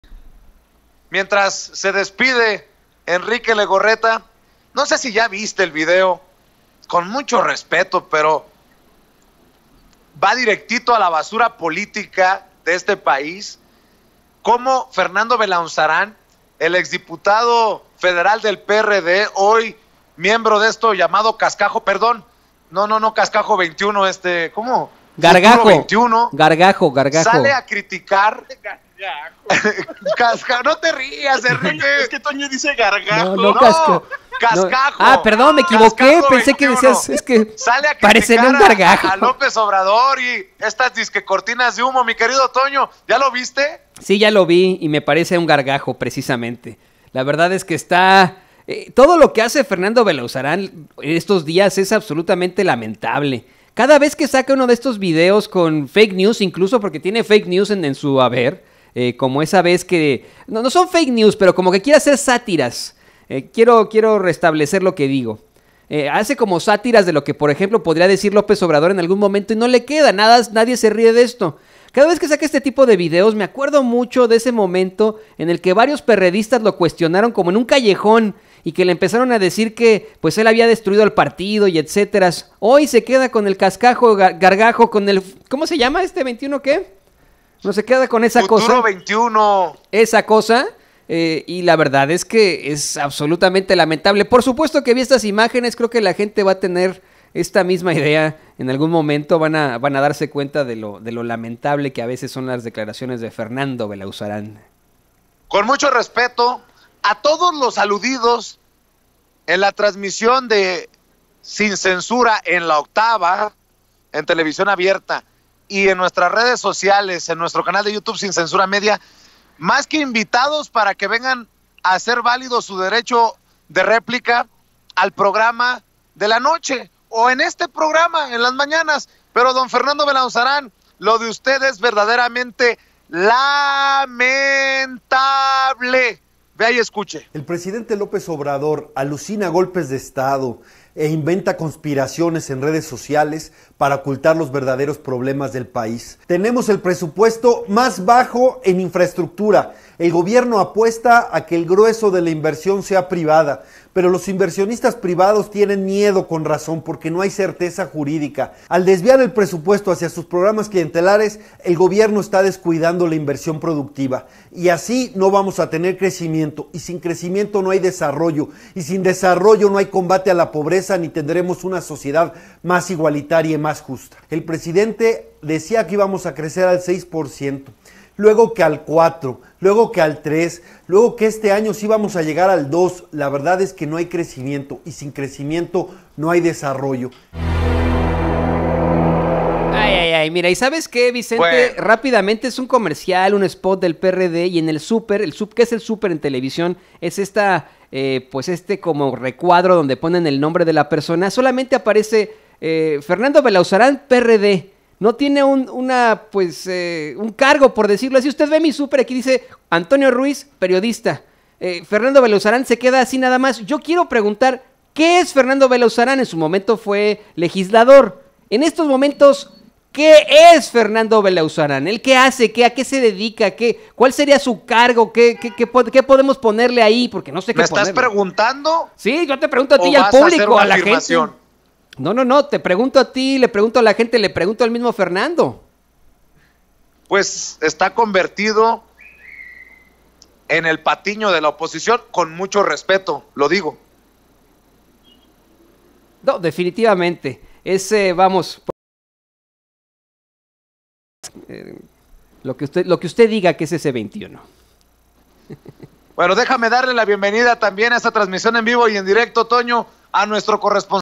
BELAUNZARÁN SACA GARGAJO Y DICE QUE AMLO ALUCINA GOLPES DE ESTADO
El (no sabemos qué es) Fernando Belaunzarán volvió a sacar un video gargajiento para golpear a López Obrador asegurando que el presidente alucina con golpes de estado